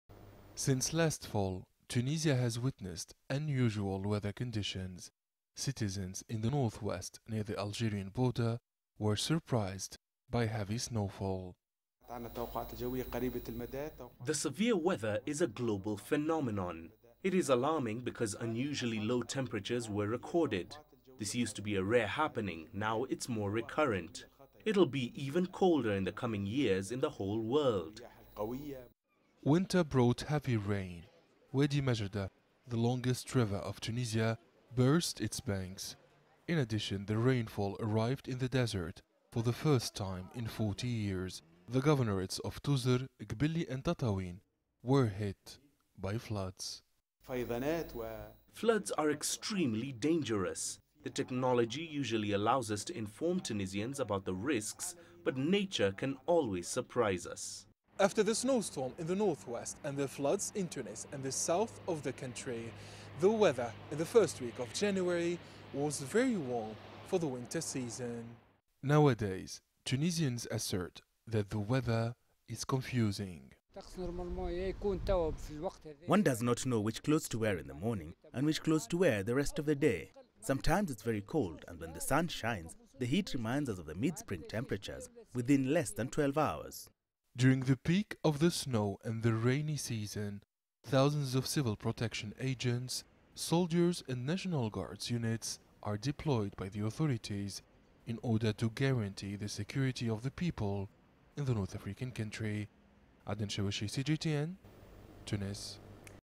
Tunisia temperature fluctuations raise concern
The current climate in Tunisia is unusual for the North African state. In just 4 weeks, temperatures have risen sharply, as CGTN's Adnen Chaouachi reports.